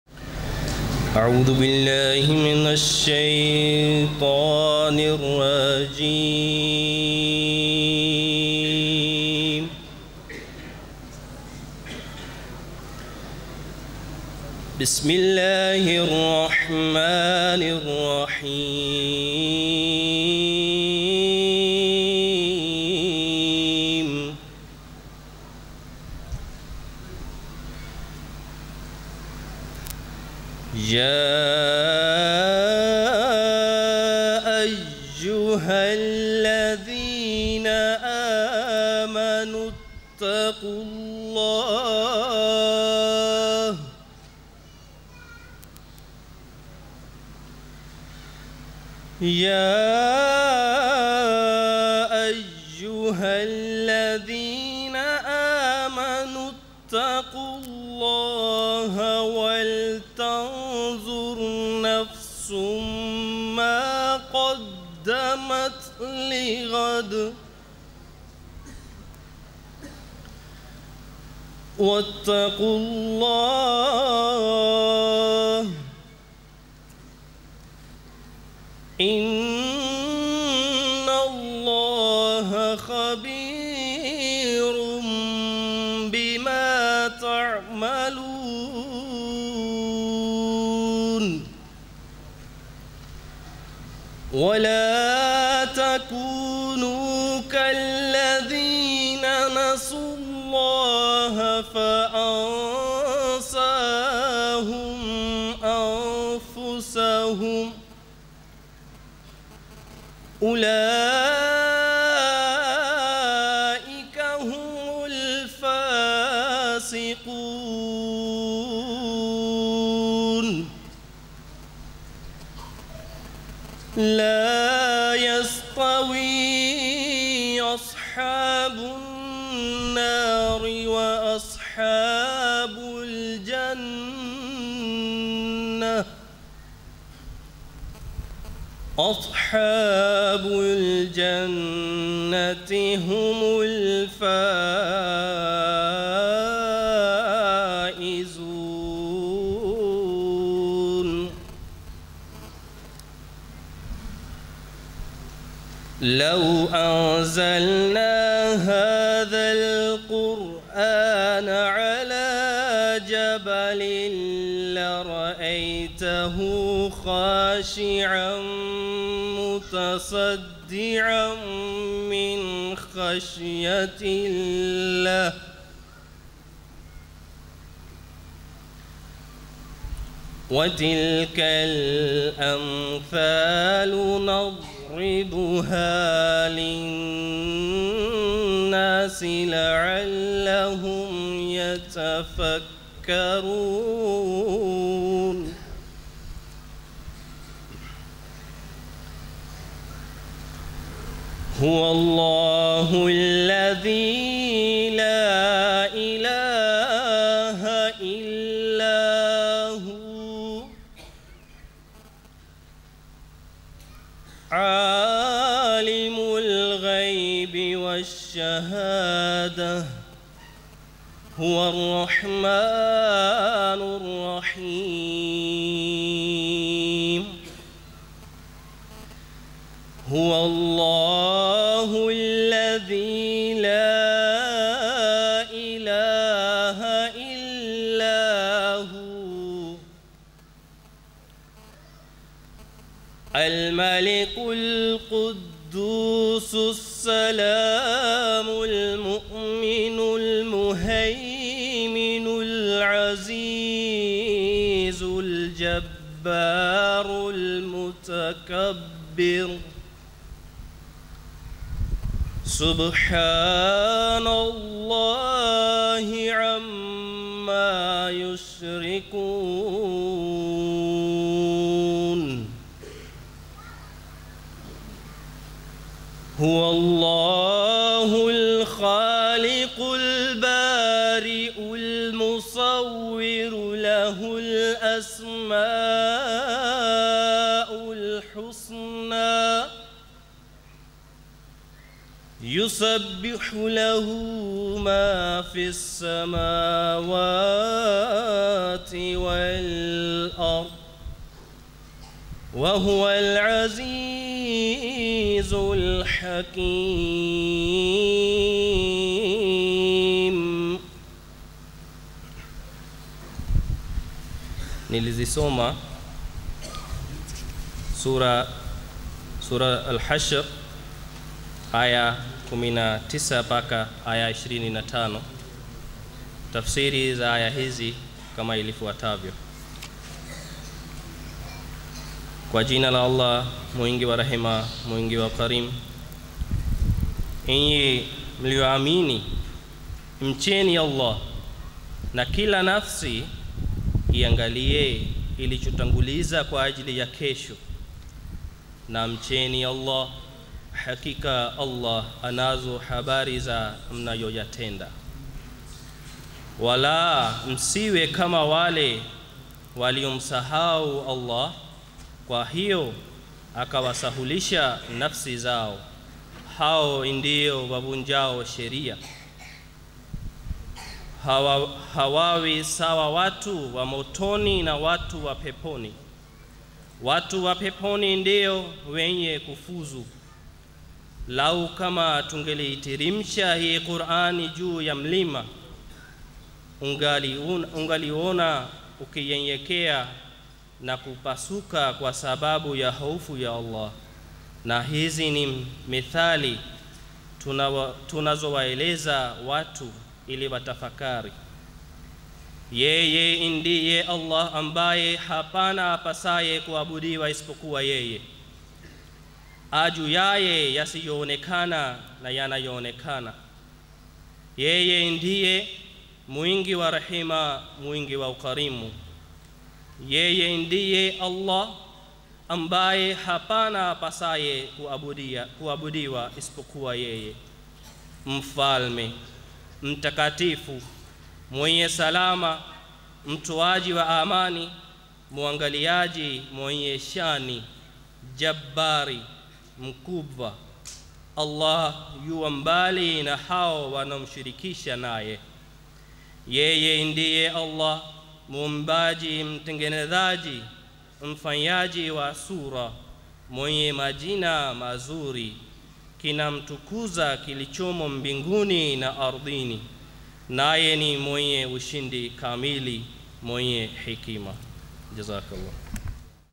Ufunguzi wa kikao cha tano kwa Quran Tukufu na Sheikh Ehtisham Latif | Jalsa Salana Tanzania 2022
Ufunguzi wa kikao cha nne cha Mkutano Mkuu wa mwaka wa Jumuiya ya Waislamu wa Ahmadiyya uliofanyika 30 SEPTEMBA - 2 OKTOBA 2022 - KITONGA, MSONGOLA, ILALA, DAR ES SALAAM kwa usomaji wa Quran Tukufu na Sheikh Ehtisham Latif sahib.
Sura ya 59 aya ya 19-25.